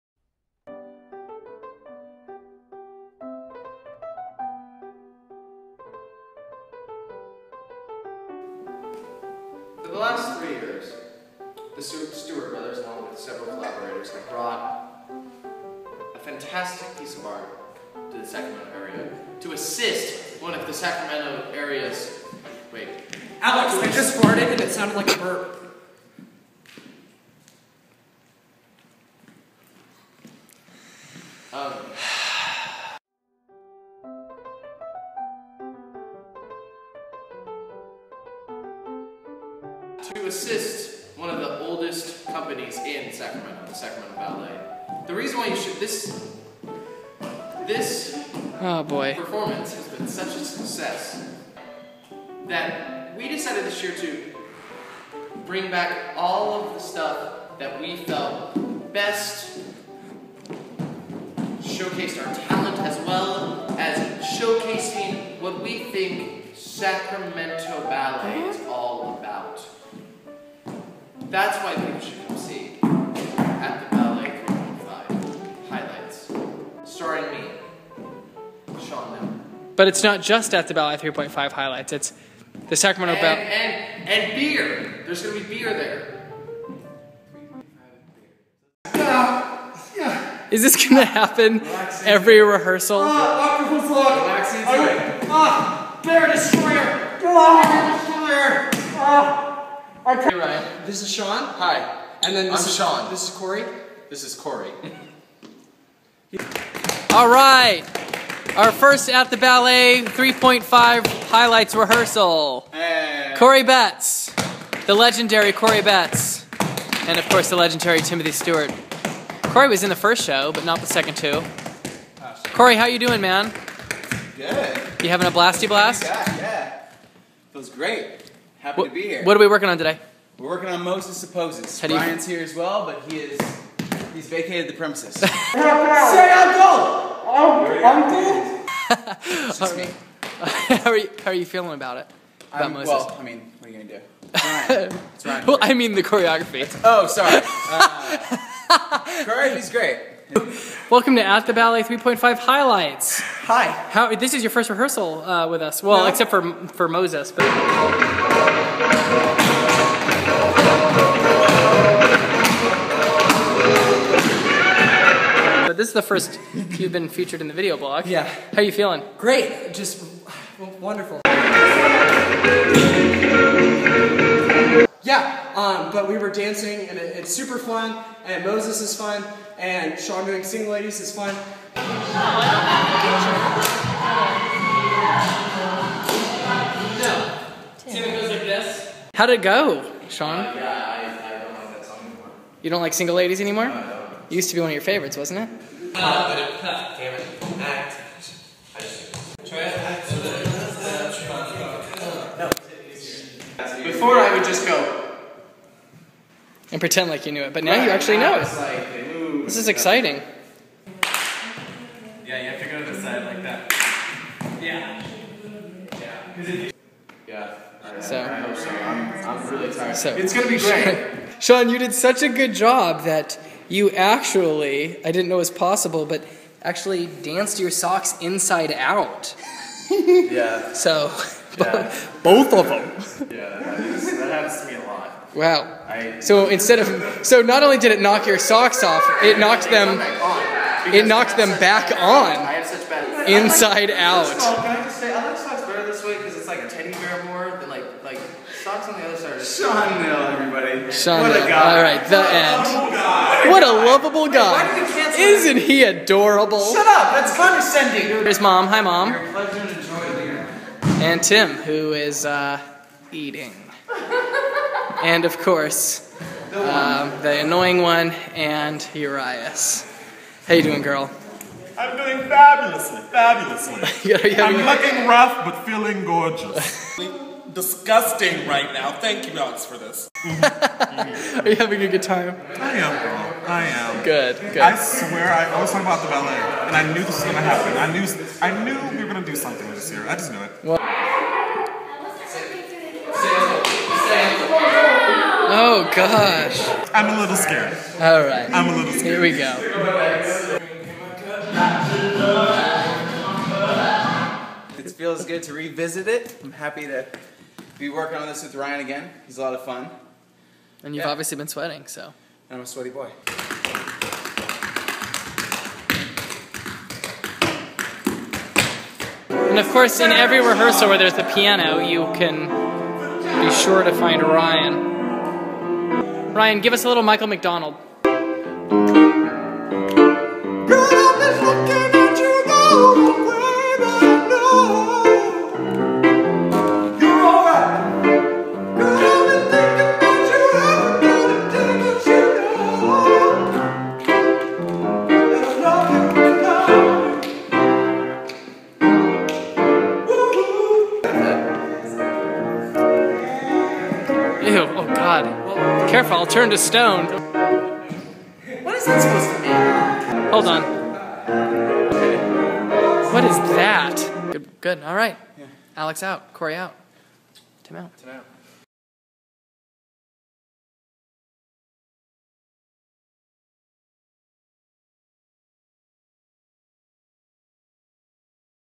At The Ballet 3.5 Vlog 1
Week 1 of rehearsals for Found Space Theatre Productions' "At The Ballet 3.5: Highlights"